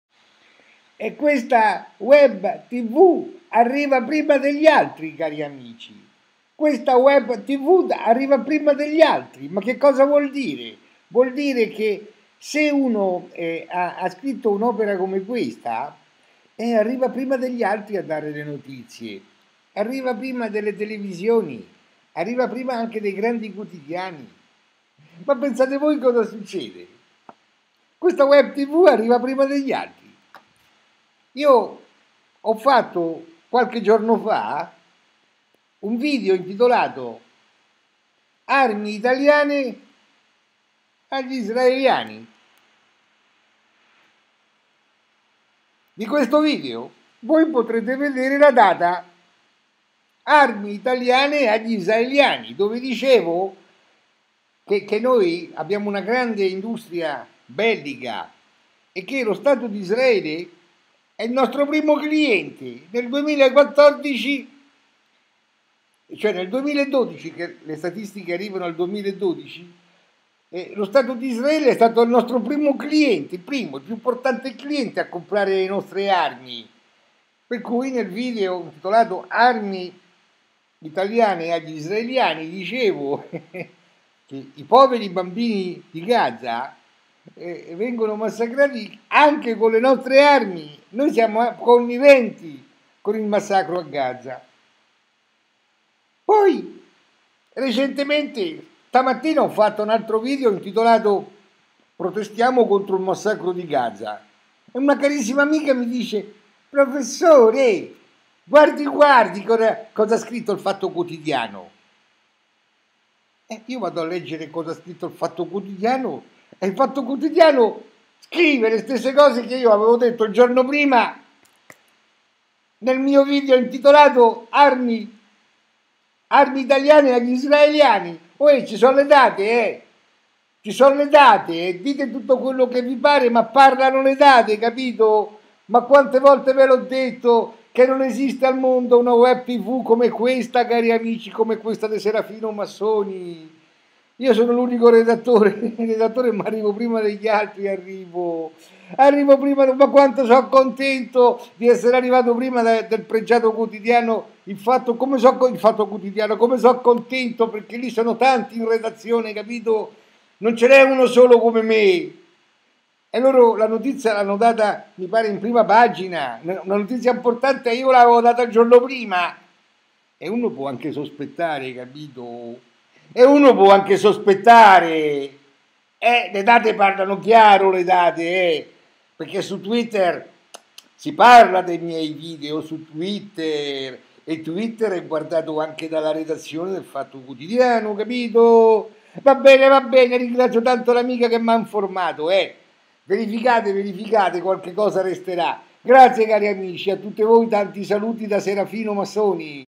Questa Mia Web Tv Arriva Prima Degli Altri Armi Contro Gaza
Comunicazione di una Amica carambolina13 ore fa

Gentile professore vorrei segnalarle questo articolo apparso oggi sul Fatto Quotidiano
Rispondi  ·  1

Serafino Massoni 59 secondi fa

+carambolina1 GRAZIE Cara carambolina, quindi posso dedurre che IL FATTO QUOTIDIANO può essersi ispirato al mio precedente video ARMI ITALIANE A ISRAELE
GRAZIE Cara Amica !!!